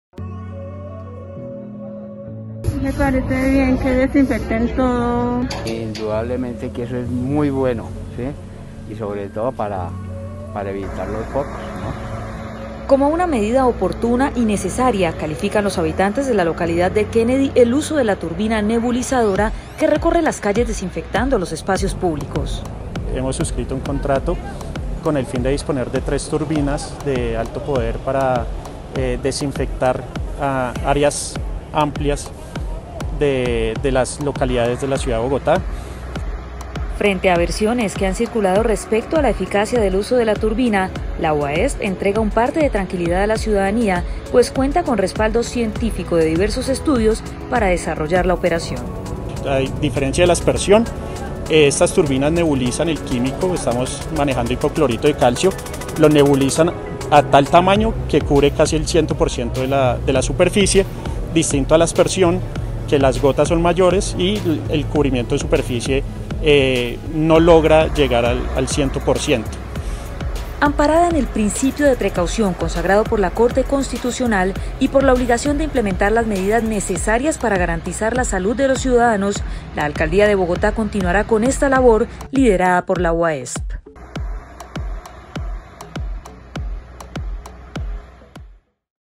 Continúan jornadas de desinfección en espacios públicos de Bogotá
Recientemente se desinfectaron vías y andenes en los barrios Kennedy Central, María Paz, Calandaima y Patio Bonito en la localidad de Kennedy en el suroccidente de la capital. La comunidad respalda los operativos